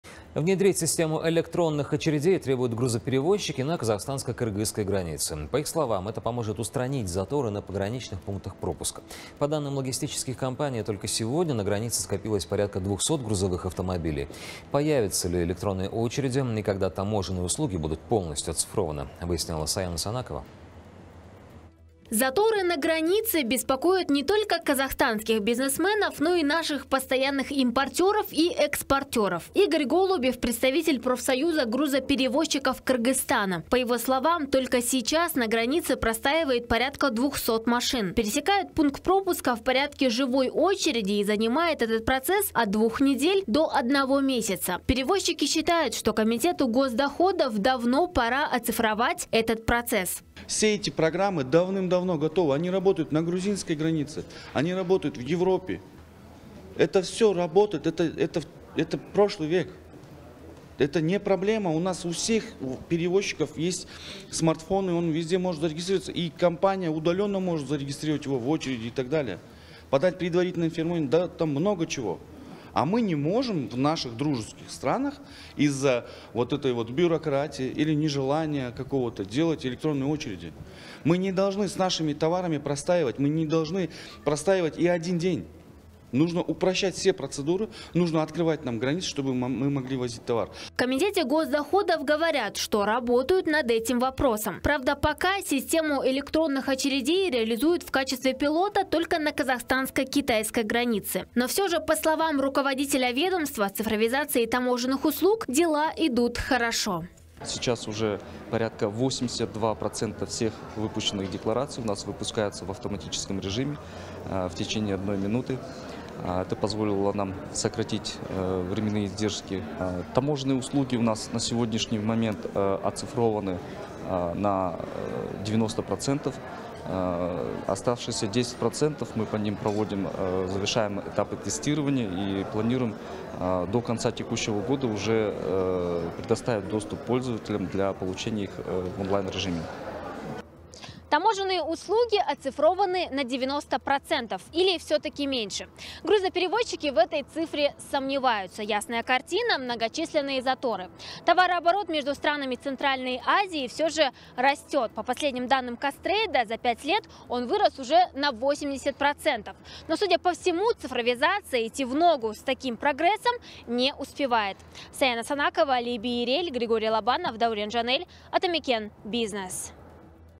ГРУЗОПЕРЕВОЗЧИКИ НА ГРАНИЦЕ ТРЕБУЮТ ВНЕДРЕНИЯ СИСТЕМЫ ЭЛЕКТРОННЫХ ОЧЕРЕДЕЙ
Внедрить систему электронных очередей требуют грузоперевозчики на казахстанско-кыргызской границе. По их словам, это поможет устранить заторы на пограничных пунктах пропуска. По данным логистических компаний, только сегодня на границе скопилось порядка 200 грузовых автомобилей. Появятся ли электронные очереди и когда таможенные услуги будут полностью оцифрованы – выясняла Саяна Санакова